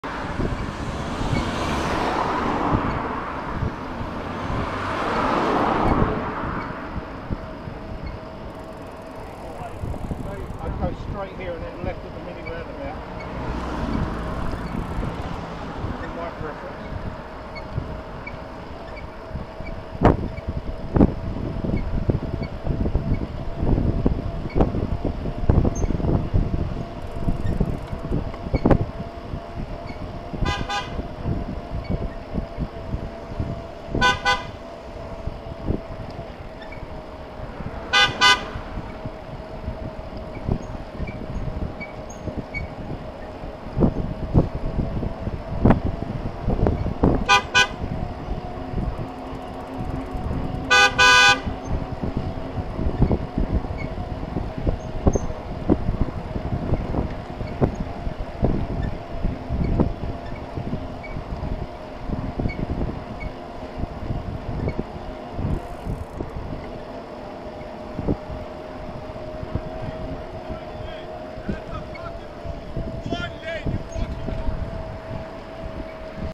KM24WTG Entitled Mercedes Driver - Intimidation and verbal abuse
Returning from an evening ride closing in on a local pinch-point, entitled Mercedes driver decides to attempt to intimidate us as soon as he gets behind. First few cars pass with no problem, but this guy begins sounding his horn and ends up shouting some unintelligible abuse.

There are quite a few Highway Code rules and recommendations about cyclists positioning, here's a few that I think are relevant:

Rule 72
Road positioning. When riding on the roads, there are two basic road positions you should adopt, depending on the situation.

1) Ride in the centre of your lane, to make yourself as clearly visible as possible, in the following situations

- on quiet roads or streets – if a faster vehicle comes up behind you, move to the left to enable them to overtake, if you can do so safely
- in slower-moving traffic - when the traffic around you starts to flow more freely, move over to the left if you can do so safely so that faster vehicles behind you can overtake
- at the approach to junctions or road narrowings where it would be unsafe for drivers to overtake you.

2) When riding on busy roads, with vehicles moving faster than you, allow them to overtake where it is safe to do so whilst keeping at least 0.5 metres away, and further where it is safer, from the kerb edge. Remember that traffic on most dual carriageways moves quickly. Take extra care crossing slip roads.

 Emphasis on the "if you can do so safely/when safe to do so". On this road there are pinch points and road furniture that doesn't allow an overtaking car to provide the recommended 1.5m passing distance. So, NOT safe.

HWC Rule 112: Never sound your horn aggressively. Difficult to say, but this definitely felt like a "Get out of my way" use of the horn. The verbal abuse after pretty much confirms this.

Honestly, if you are that angry from being delayed by probably no more than 10 seconds, perhaps you shouldn't be behind the wheel of a car...

BTW, the sqeaking was not my bike!!